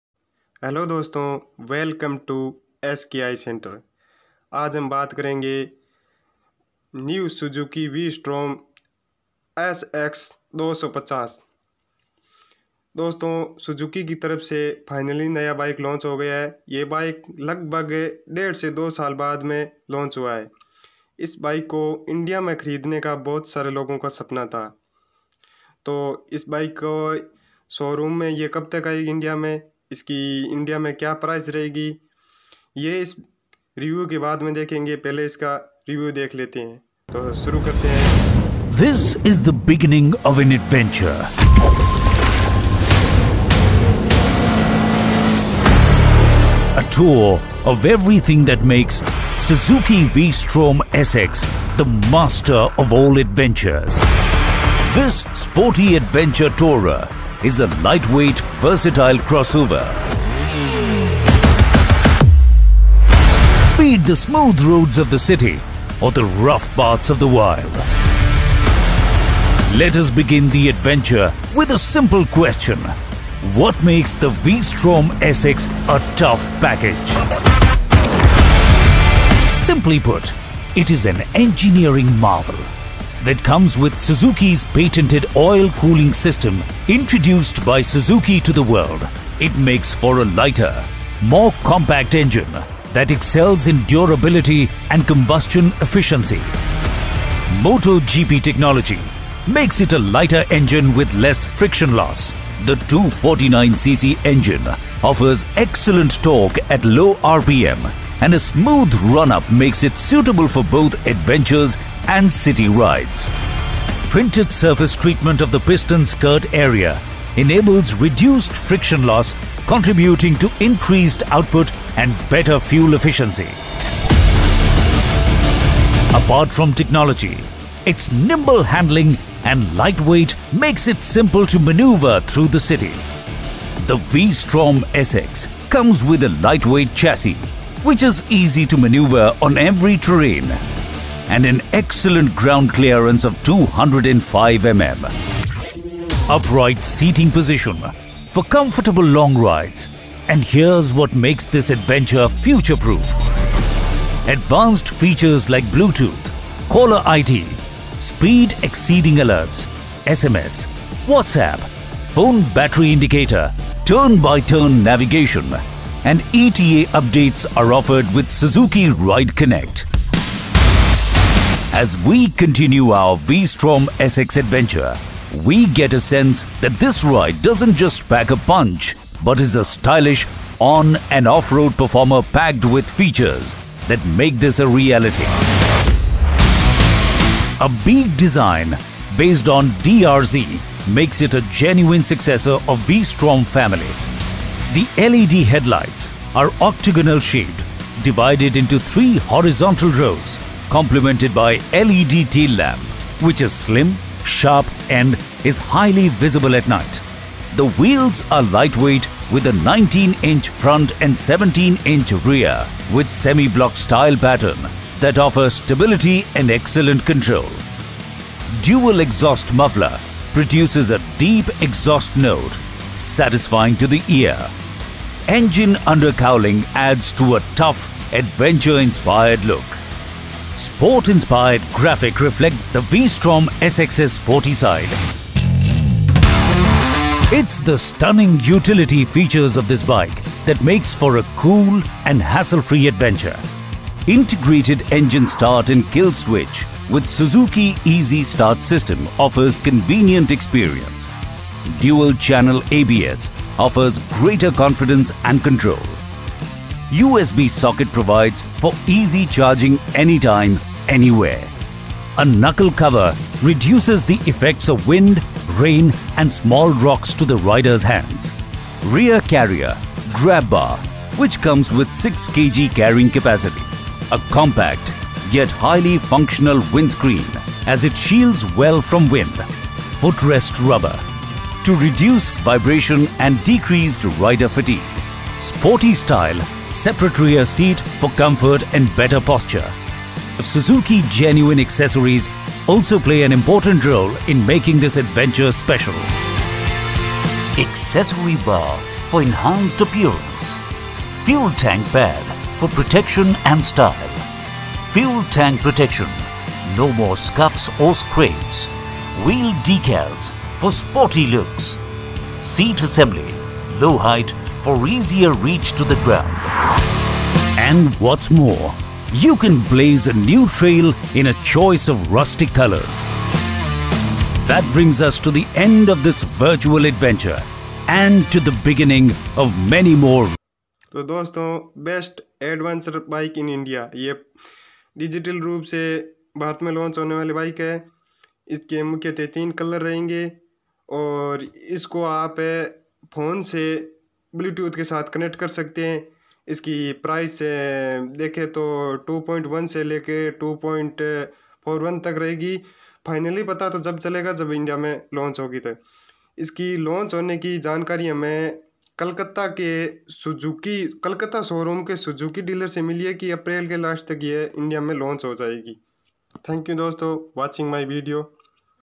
Suzuki V-strom250
1.Is Suzuki V-Strom reliable?
2. Suzuki V-Strom 250 review
   दोस्तों बहुत सारे लोगों का सपना था मारुति सुजुकी की बाइक खरीदने का जो आज पूरा होने जा रहा है जी हां मारुति सुजुकी ने बेस्ट एडवेंचर बाइक इन इंडिया जो न्यू सुजुकी v-strom  sx250 लॉन्च की है जिसकी प्राइस 2.11 है इस बाइक में आप अपने फोन से ब्लूटूथ के साथ कनेक्ट कर सकते हो। इंडिया में आने की जानकारी कोलकाता के शोरूम मारुति सुजुकी के डीलर से मिली है जो उन्होंने कहा है अप्रैल के लास्ट तक यह बाइक हमारे शोरूम पर आ जाएगी।

   Friends, many people had a dream of buying a Maruti Suzuki bike, which is going to be fulfilled today, yes, Maruti Suzuki has launched the best adventure bike in India, which is the new Suzuki v-strom sx250, which costs 2.11, in this bike you can buy your phone.  Can connect with bluetooth.  The information about coming to India has been received from the dealer of Maruti Suzuki showroom in Kolkata, which they have said that this bike will arrive at our showroom by the end of April.